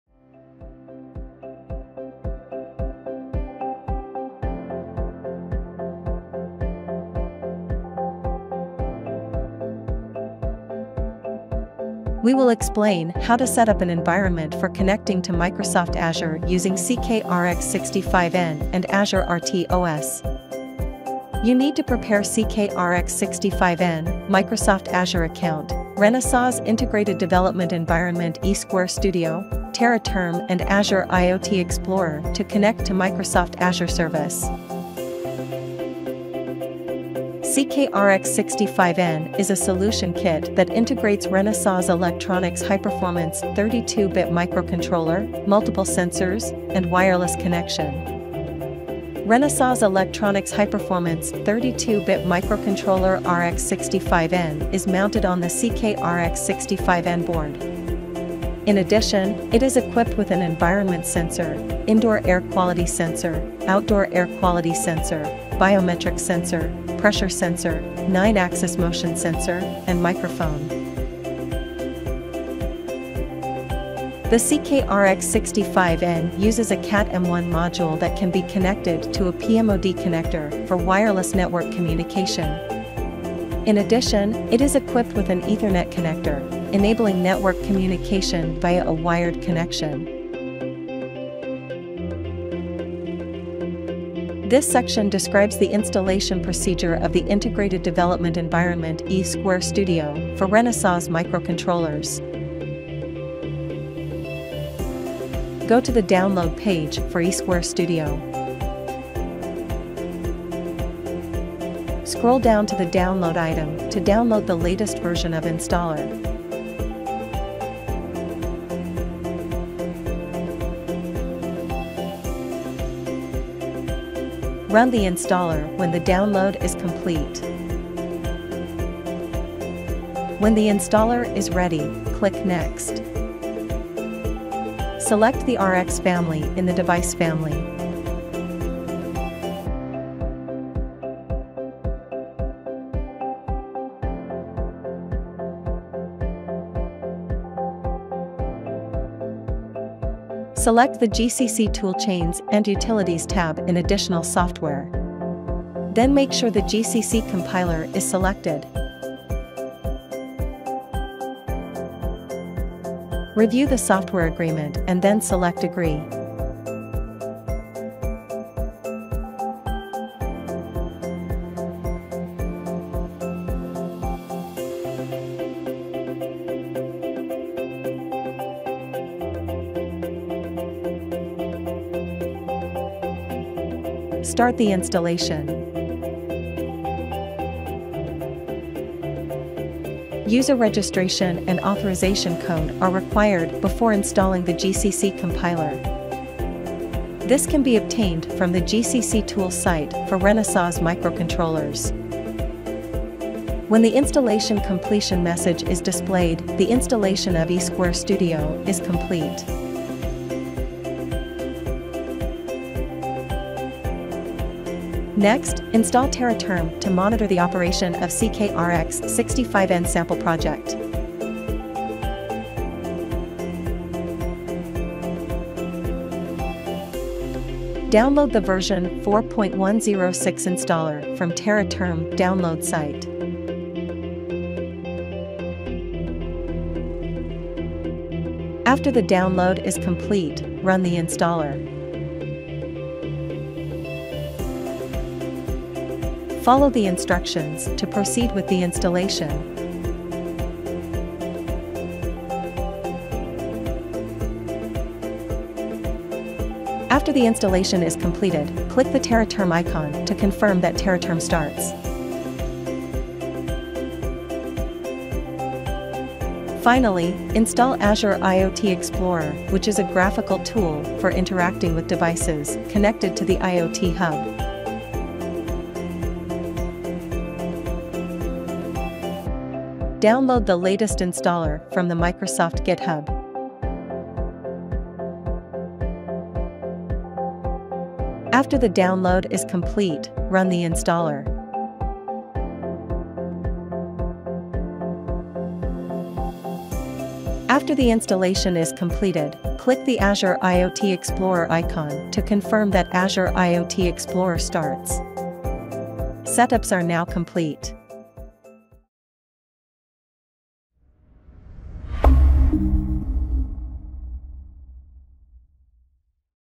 Azure RTOS Tutorial (1/3) CK-RX65N: Setup
This video series shows how to use Azure RTOS with the CK-RX65N